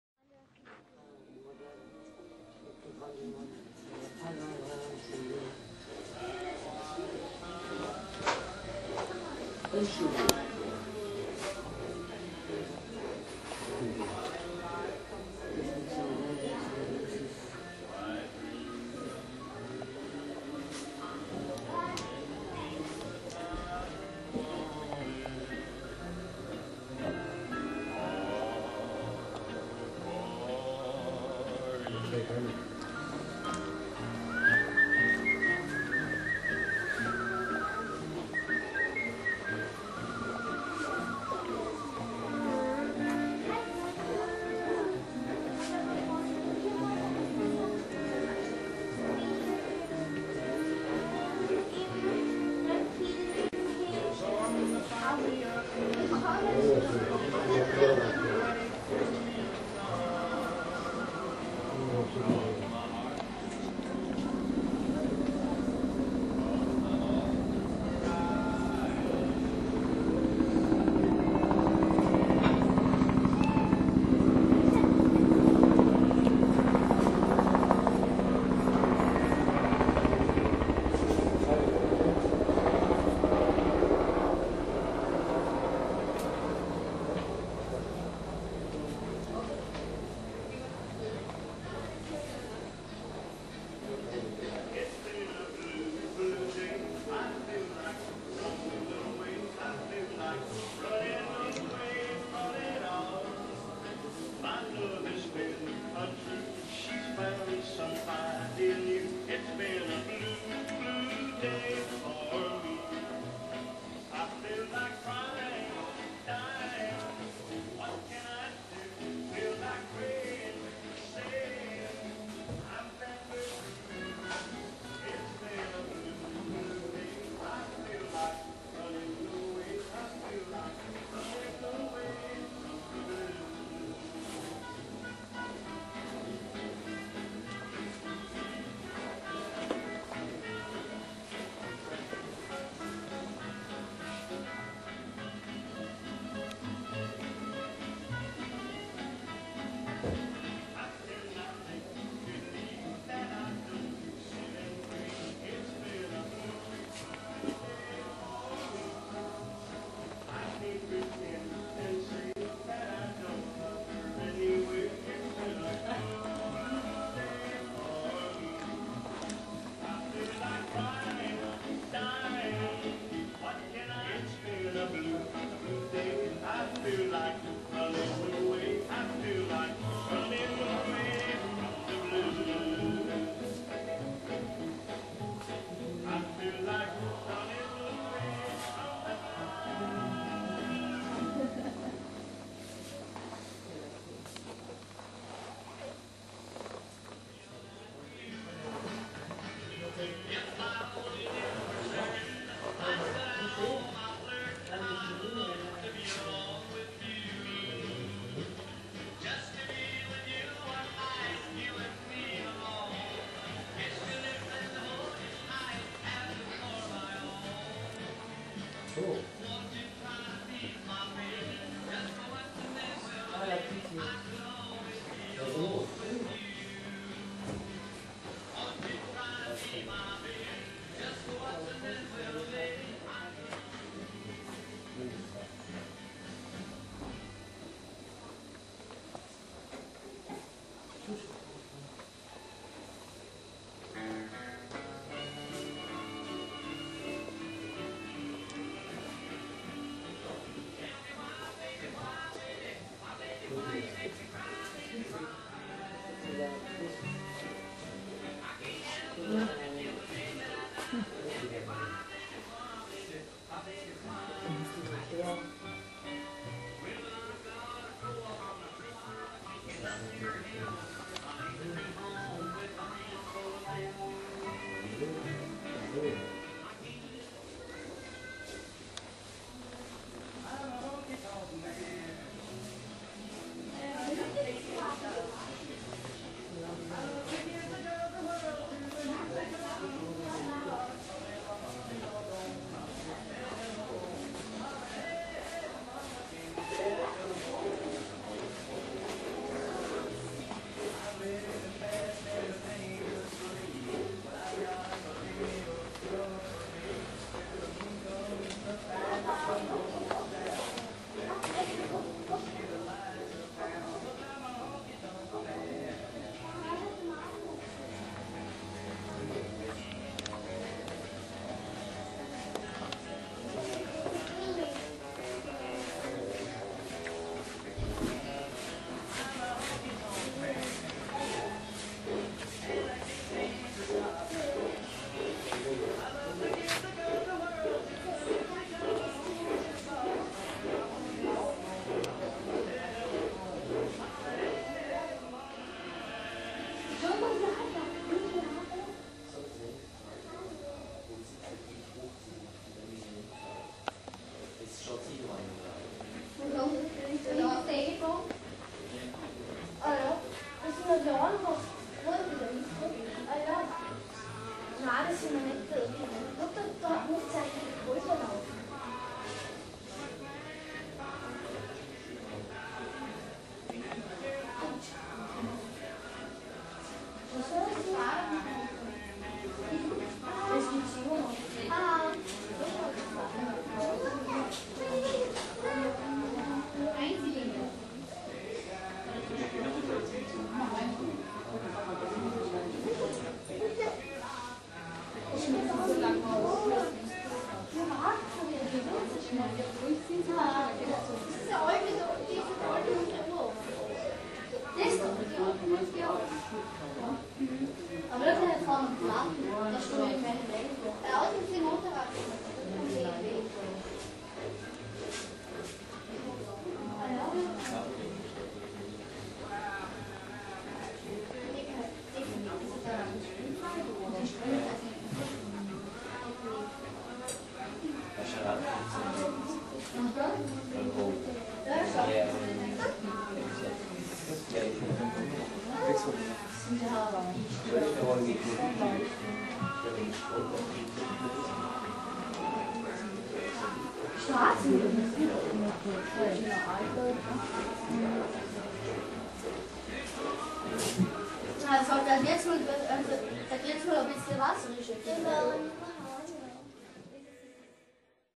Motorrad Ausstellung ( Mostra di Motociclette )
Sipplingen - Bodensee ( Lago di Costanza ) Germany
Motorrad Ausstellung mit Modellen von 1920 bis 1988